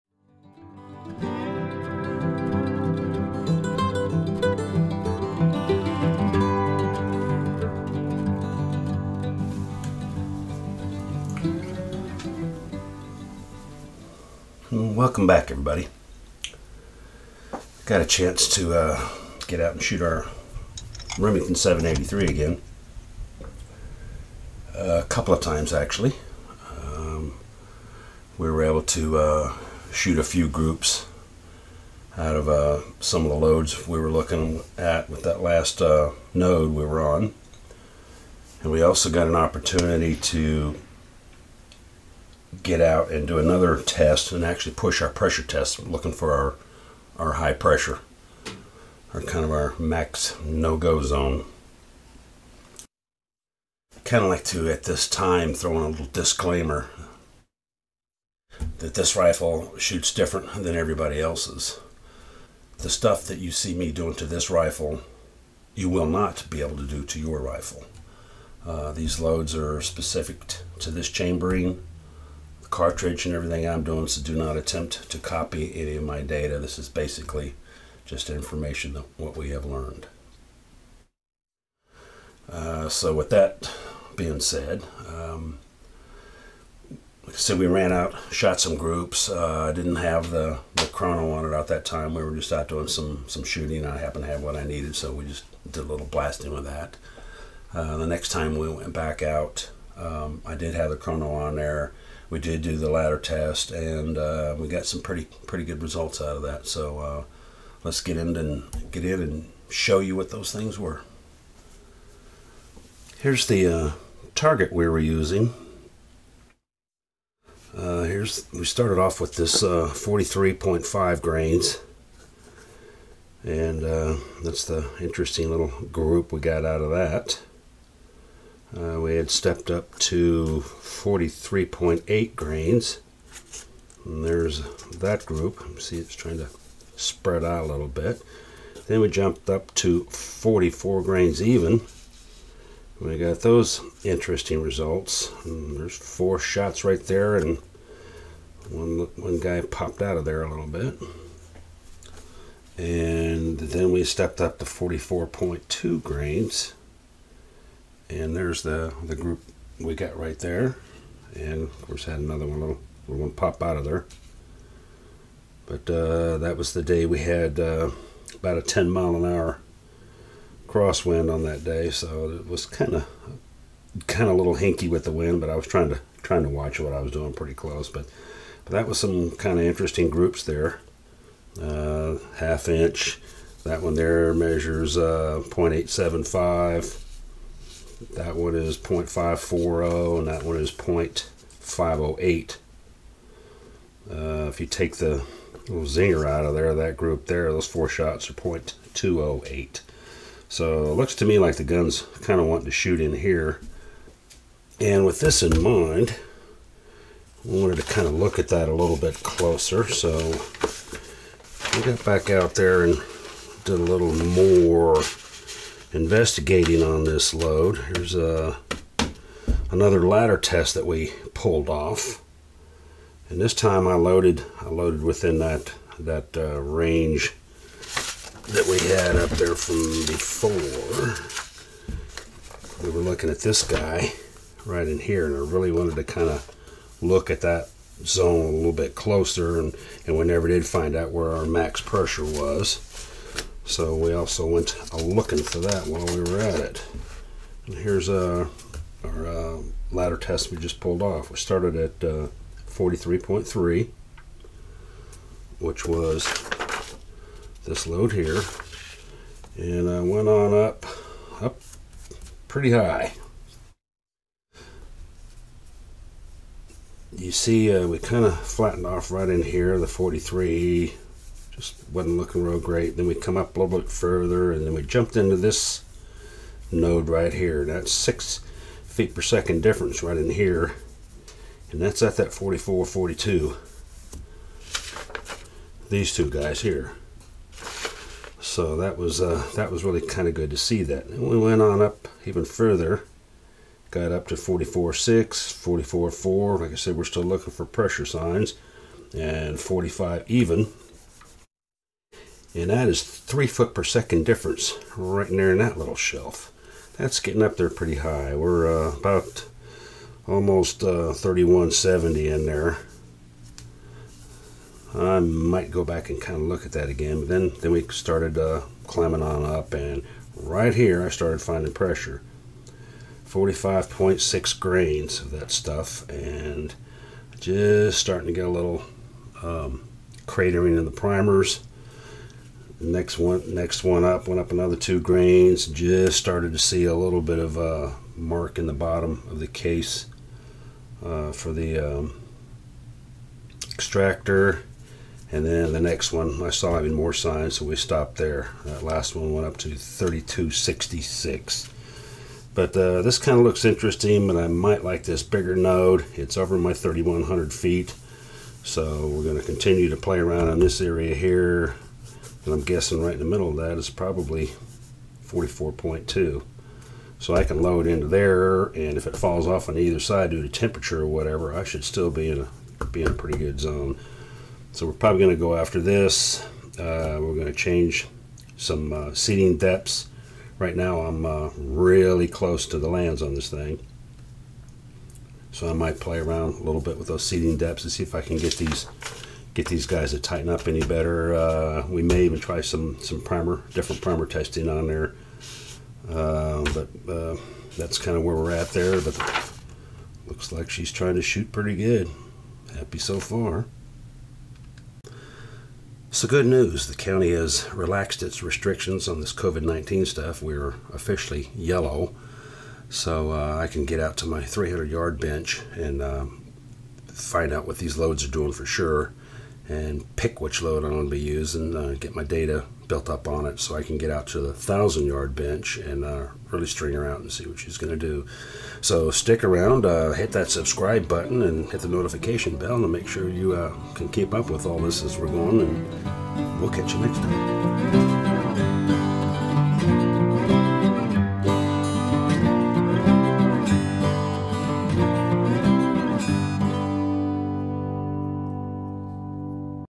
Ladder Testing the 783 and Max Pressure Found
Found 2 pressure nodes that look great and the Danger zone as well.

Remington 783 in 243 Winchester
Berger 105 Hybrid bullets
IMR 4350 powder
CCI 250 primers
Winchester brass
Oryx Chassis short action
X-Caliber Barrel 1-8" twist with 3r rifling
Nikon FX1000 scope 6-24x50 MIRAD
Swiss Arca rail from Oryx  #104595-BLK
Tool-less Wheel Knob from Oryx #105124-BLK